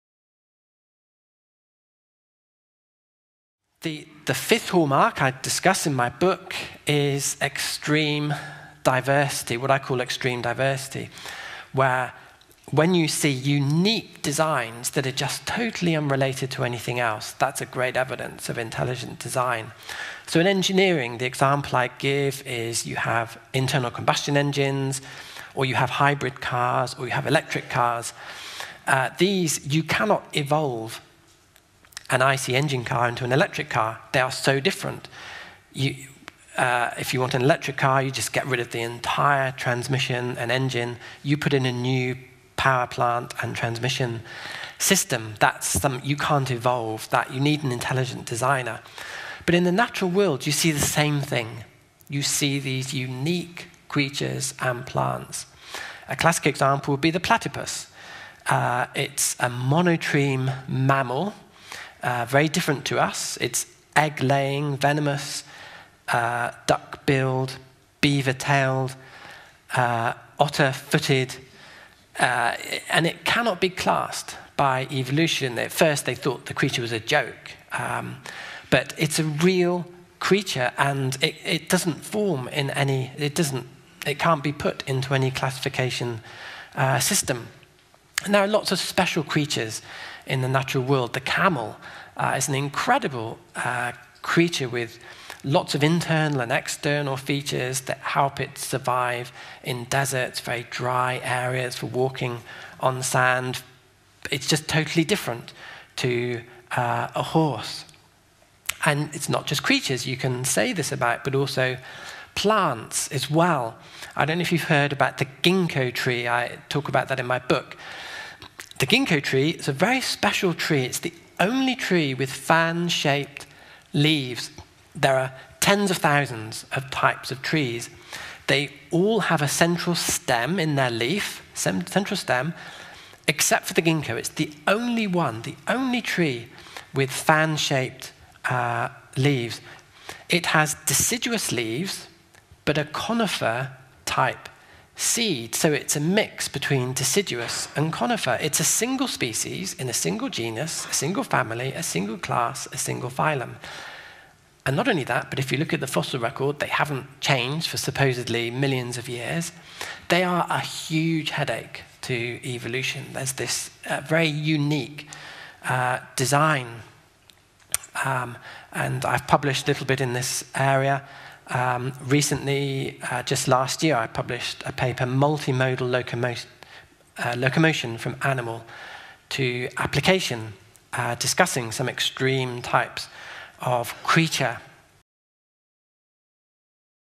Hallmarks of Design. 5. Extreme Diversity. Professor Stuart Burgess. Creation Evidence.
Professor Stuart Burgess explains that there is extreme diversity in creatures, which could never evolve one into another, and gives the Darwinian evolution theory a huge problem. Engineers know that the internal combustion engine is so different from the hybrid and electric cars one couldn’t ‘evolve’ into another. Yet there are many unique creatures of which the same could be said, for example the platypus, camel and Ginkgo tree.
Professor Stuart Burgess was awarded the IMechE James Clayton Prize for the biggest contribution to mechanical engineering science in the UK in 2019. He is presently Professor of Engineering Design at Bristol University.
Profstuartburgess.com